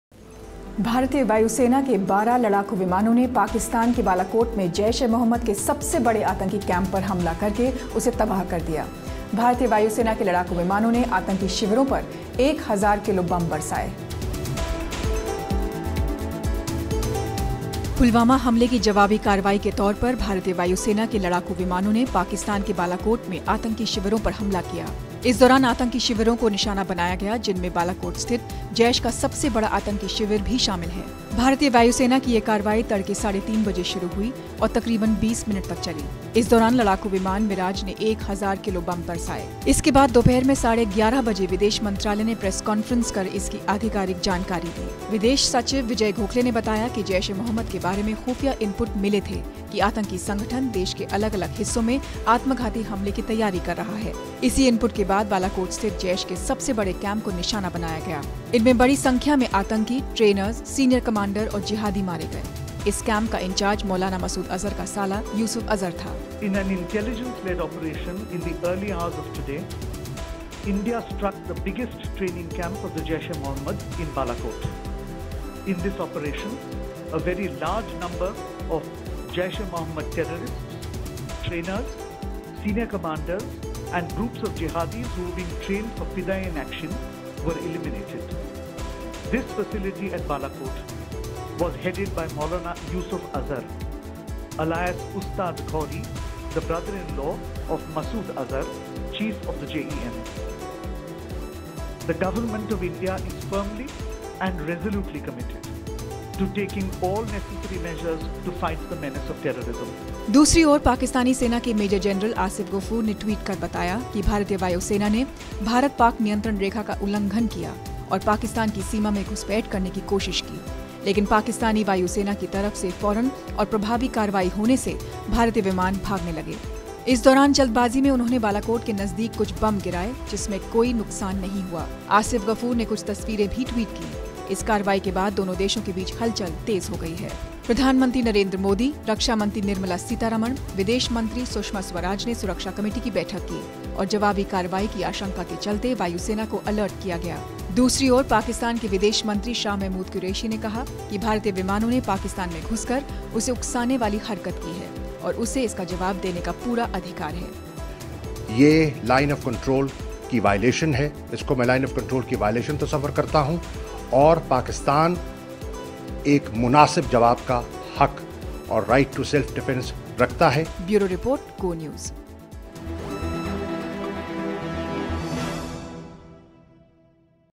IAF Strikes Jaish Camp In Pakistan
Tensions are high after 12 IAF Mirage fighter jets destroyed the main Jaish terror camp in Balakot across the LoC early on Tuesday. Pakistan has warned it has the complete right to self defence